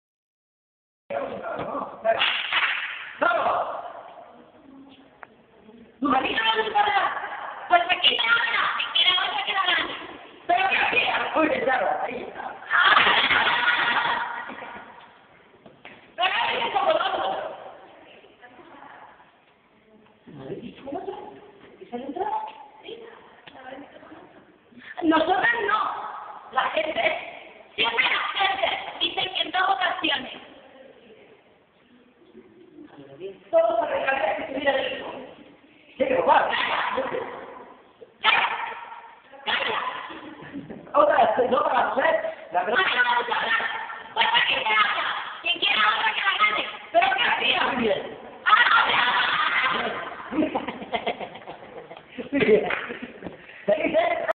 I Taller de Teatro Físico - ensayos
Prácticando la voz durante el I Taller de Teatro Físico impartido por Nekalá Company (Raúl Vargas e Ifigenia Androutsopulus) durante la semana del 5 al 10 de noviembre de 2012 en La Nave de Los Arcos de Motril.

El taller, organizado por Alifa.org y el CSA La Fabriquilla de Ideas, culminó con la representación de una adaptación libre de "Yerma" de Federico García Lorca.